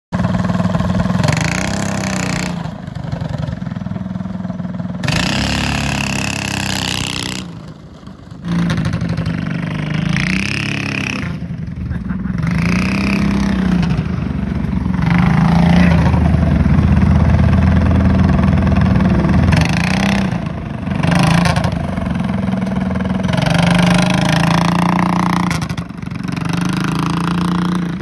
Dodge Van Go Kart with Reverse gear
Custom go kart, vintage frame, updated with go fast parts and an original FW Associate van go kart body